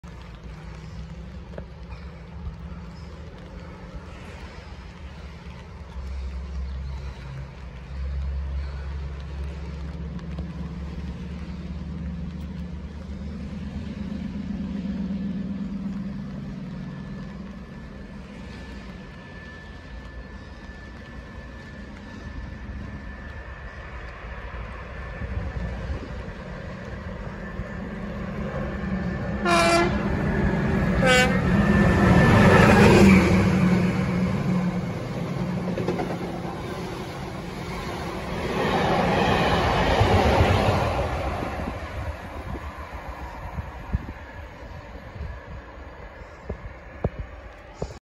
The ECS movement of the deltic royal Scot’s grey special tour April 21st 2023
Switching counties now and I’ve come to the north Warwickshire market town of atherstone for the first time in 5 years and shame the weather is as miserable as it was when I attended the spring gala on the Severn valley railway

Anyway here’s my lucky opportunity to see royal Scot’s grey now back to its normal guise and carrying its old number again d9000 and carrying the green livery for the first time in 21 years the last time it wore was back in 2002 back then when it was running with the D9000 fund

The train is seen whizzing through atherstone station on the slow line heading with the Scotrail coaches carrying the golden eagle headboard heading for its former stomping ground terminus of King’s Cross in readiness of its tour of today to Edinburgh and occupied at the back of the train is the intercity class 90 loco of 90001